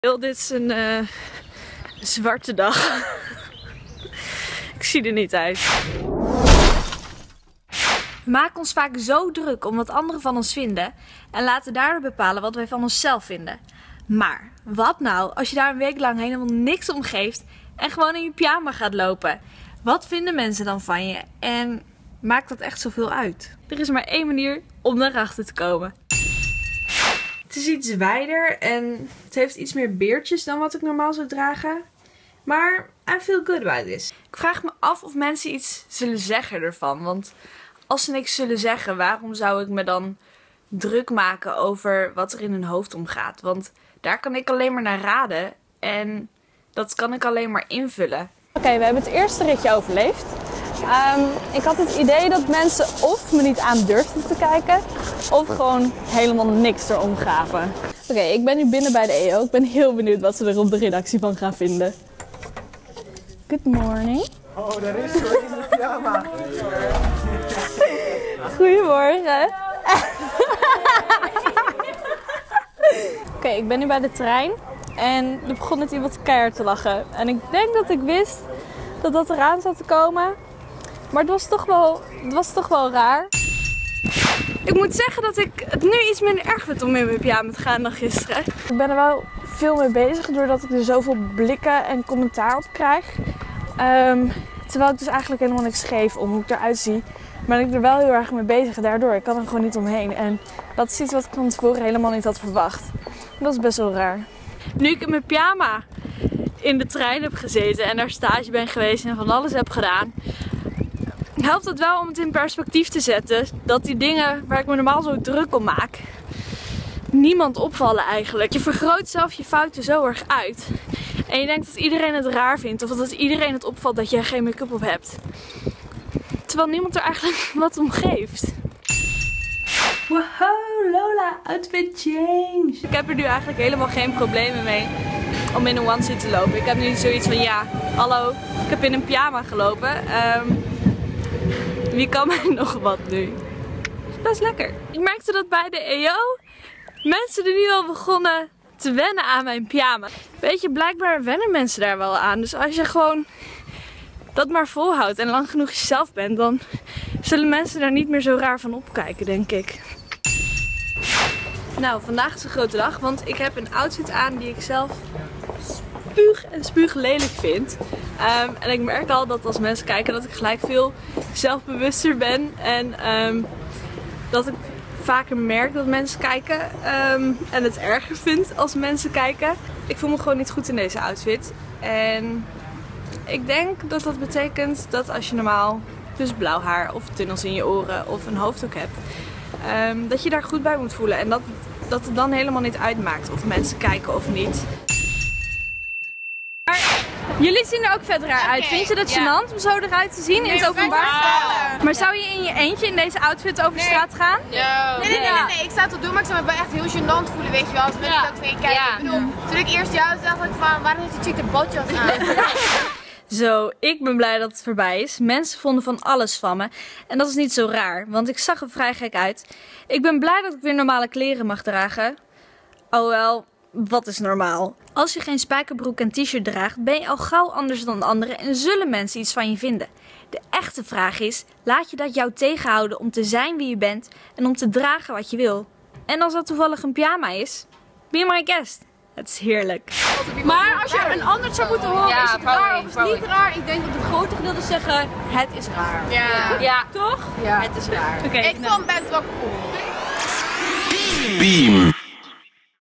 BEAM Test: Een week lang in je pyjama!
Lola loopt een week lang in haar pyjama! Hoe voelt dat en hoe reageren mensen daarop? Spannend!